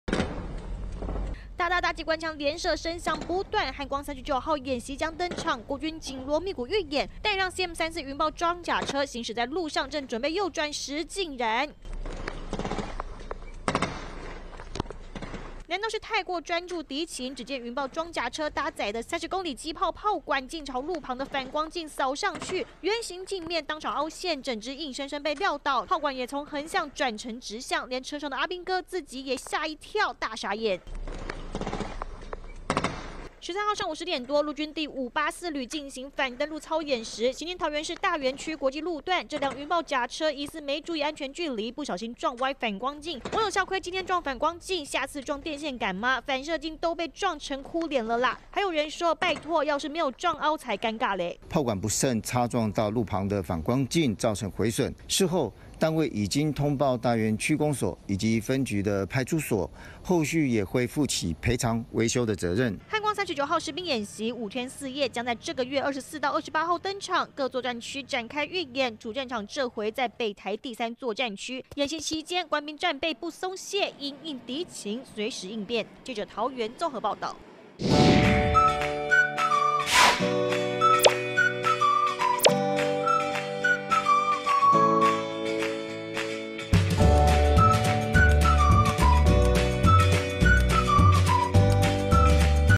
【每日必看】尷尬了...漢光演習預演 雲豹甲車砲管"撂倒反光鏡" 20230714
國軍漢光39號演習7/24將登場，本週起各作戰區也展開預演，陸軍584旅執行反登陸及戰甲車戰鬥搜索時，被民眾拍到一輛雲豹八輪裝甲車，行經桃園大園區，砲管不慎擦撞路旁反光鏡，將反射鏡撞爆圓臉當場變哭臉。軍方也通報當地區公所及警方，聯繫後續賠償。